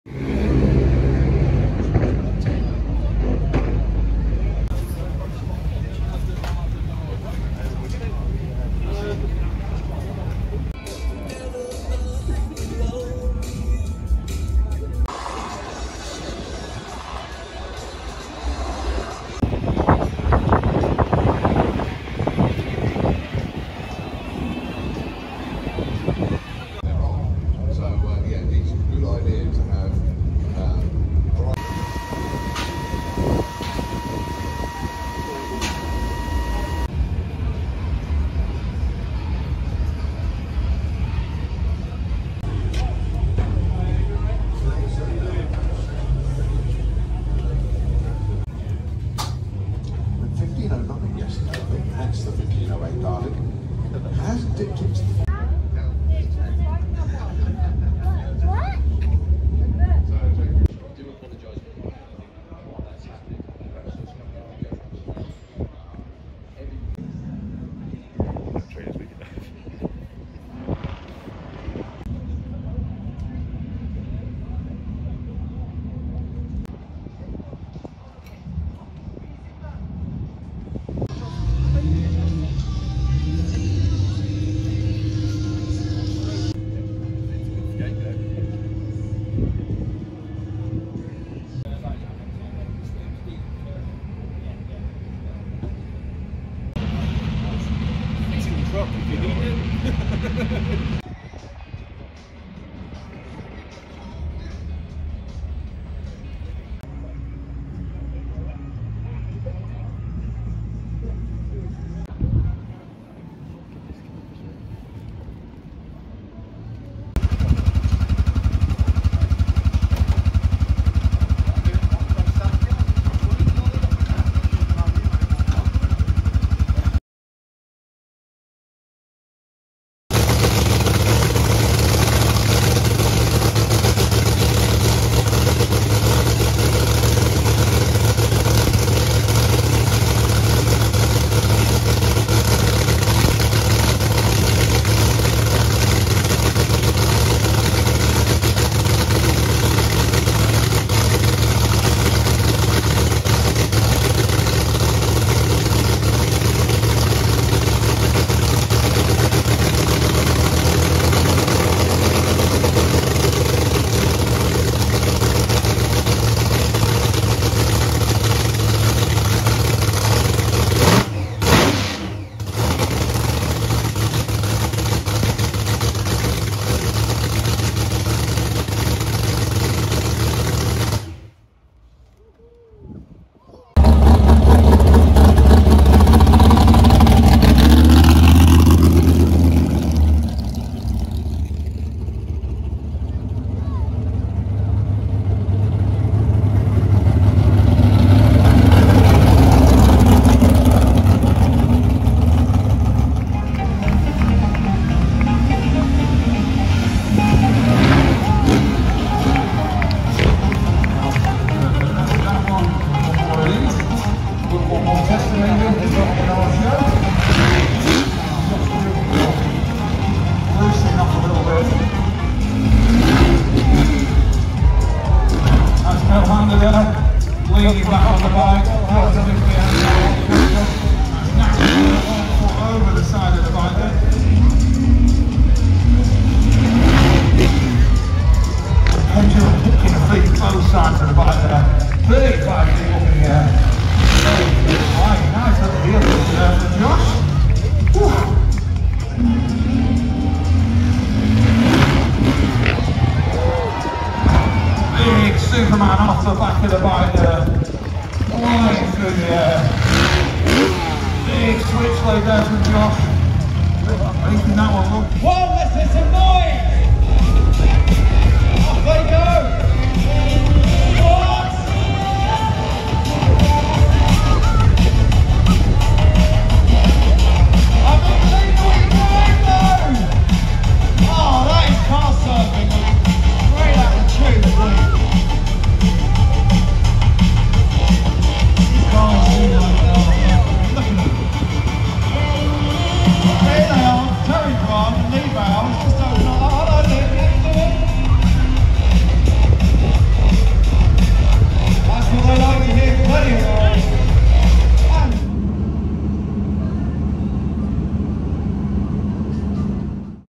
Festival of Power 2024 | Around the pits/off-track action | Santa Pod
A look around the pits at some of the race machinery parked up while on a rain delay at the Festival of Power, and also at some of the action in the arena by the viewing bank.